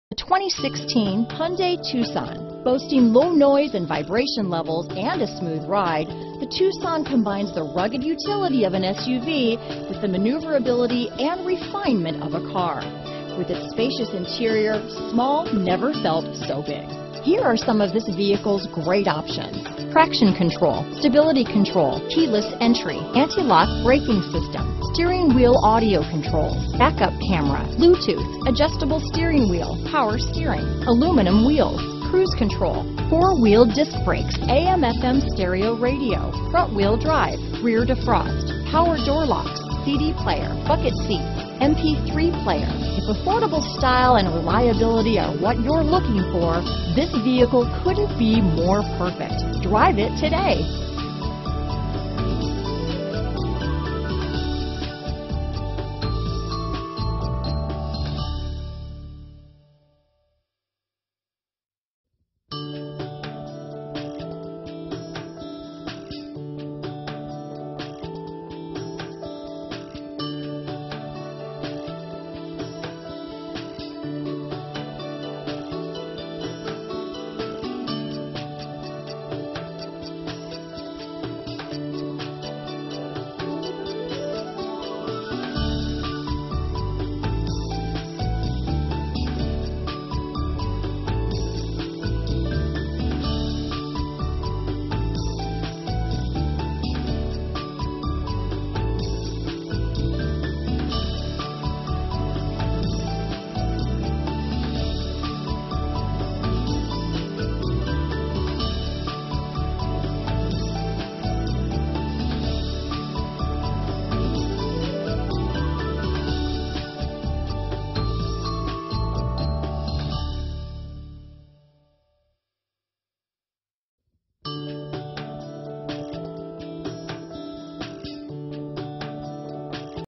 2016 Hyundai Tucson Orange County, Irvine, Laguna Niguel, Newport Beach, Mission Viejo. H8678
2016 Hyundai Tucson

Allen Hyundai
28432 Camino Capistrano

SE Popular Package 02 (8-Way Power Driver Seat Adjuster, Driver Power Window w/Auto Up/Down, Dual Illuminated Vanity Mirrors, Fog Lamps, Illuminated Glove Box, LED Daytime Running Lights, Premium Side Sills, and Roof Rack Side Rails), Tucson SE, White, and Gray.There isn't a nicer 2016 Hyundai Tucson than this fuel-efficient gem. Climb into this gas-saving Tucson for a smooth ride with the kind of economy that puts a smile on your face.Price includes: $500 - HMF Bonus APR Cash - Standard - CE/SC/SO/WE. Exp. 08/01, $500 - Retail Bonus Cash - National. Exp. 08/01